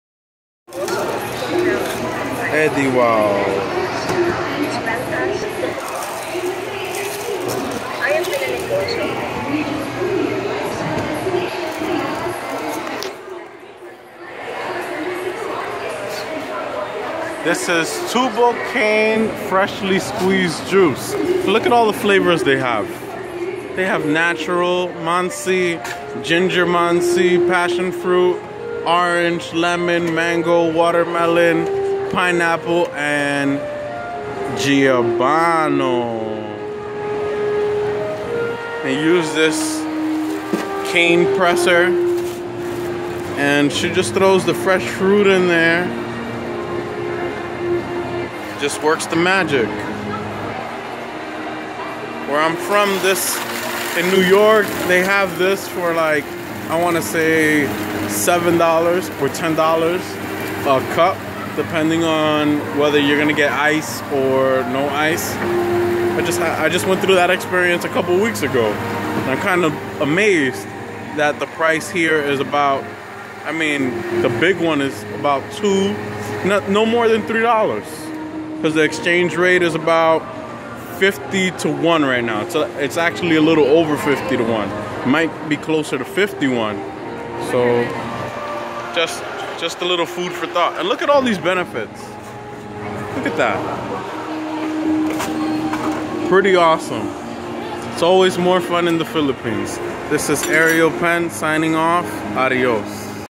Tubo Cane Juice - Cebu Ayala Mall
I found this place and was very interested by the flavors, Ive seen Cane pressed like this for its juices before but I have never seen a mix of the Cane juice with a variety of other juices before. Absolutely delicious and awesome. Check it out.

Its always more fun in the Philippines.